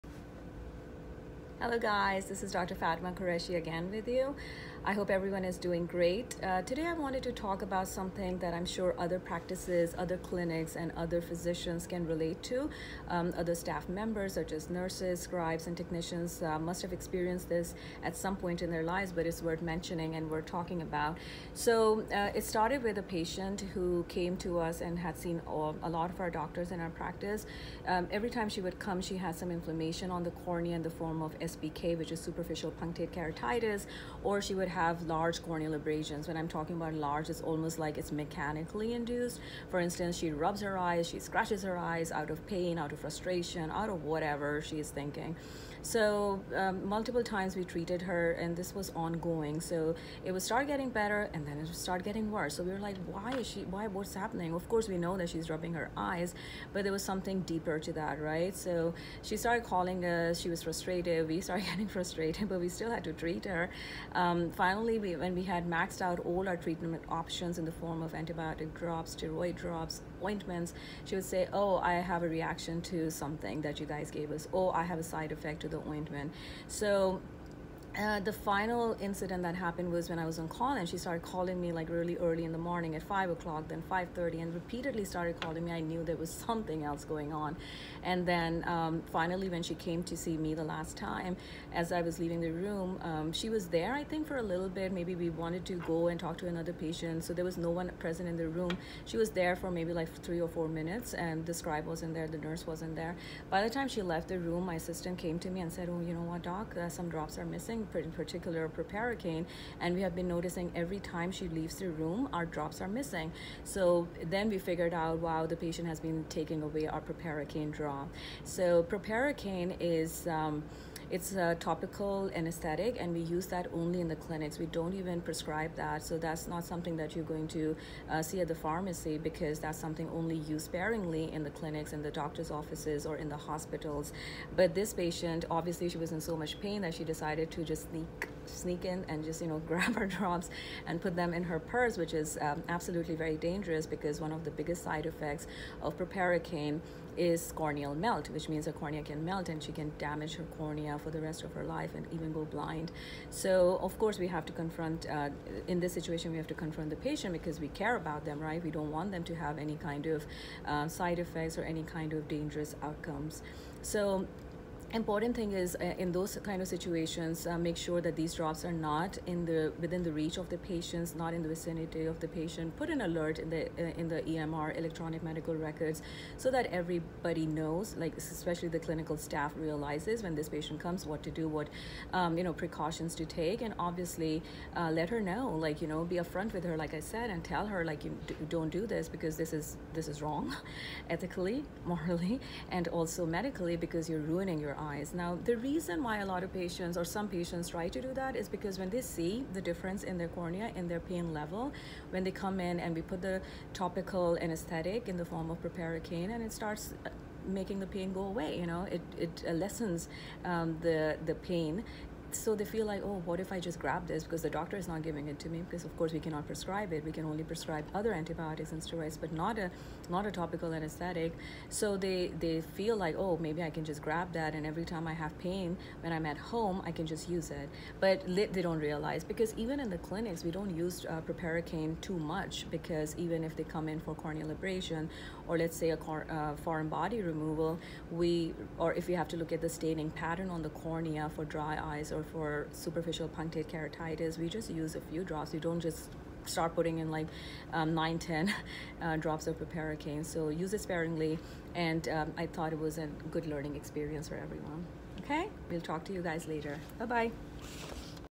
Proparacaine abuse - do not use any eye drops without the doctors permission.
Listen to how patients can damage their cornea with topical anesthetic drops.